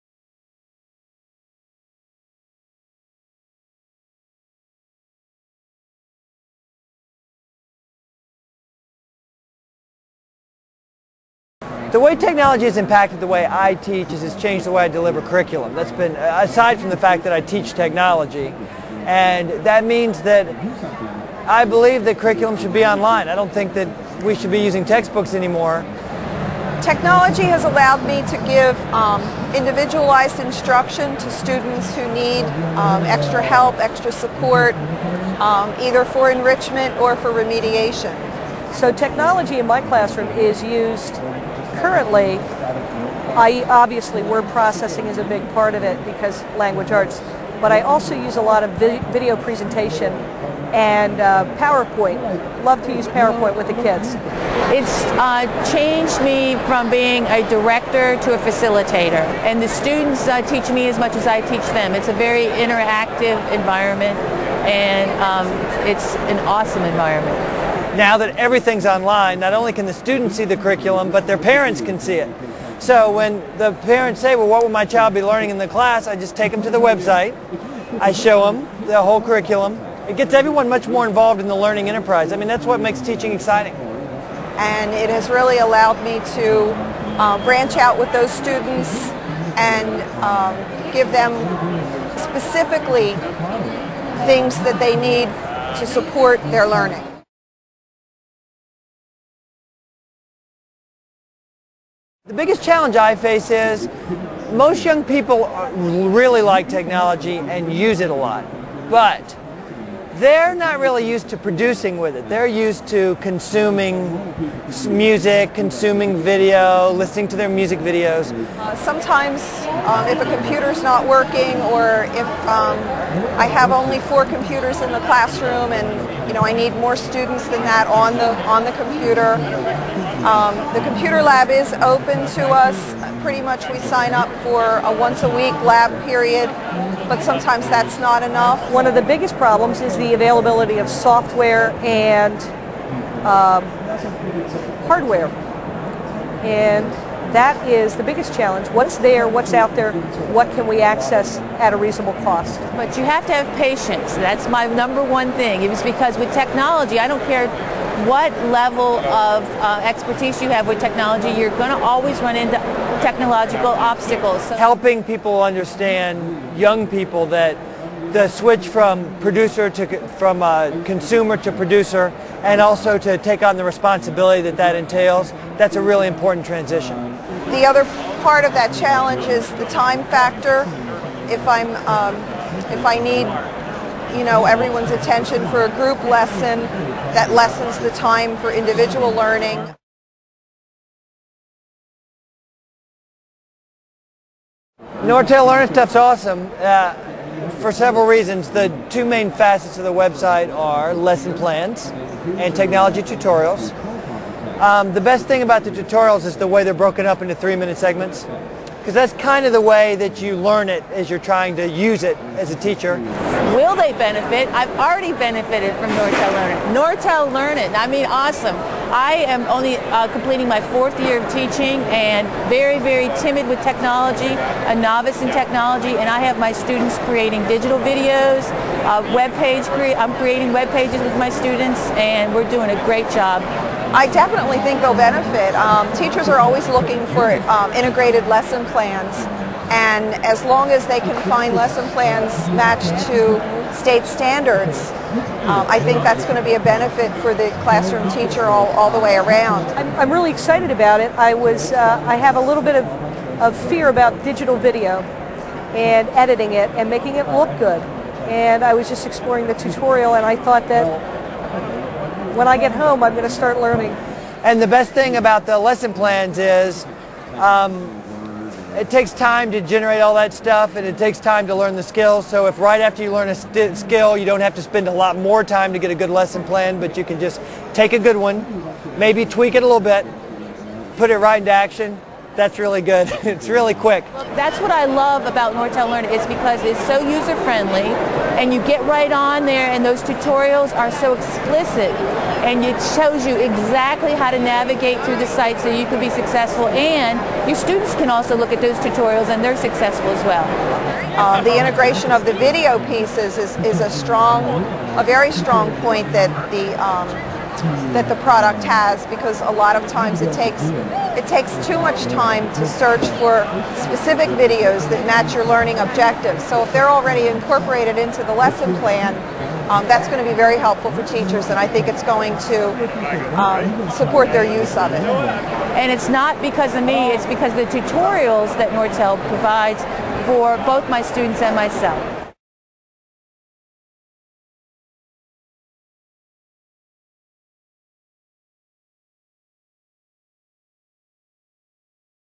Talking Technology:2005 NECC Floor
Hear teachers on the floor of the 2005 NECC Convention describe the role technology plays in their teaching and how Nortel LearniT helps them succeed in their 21st century goals.

Nortel LearniT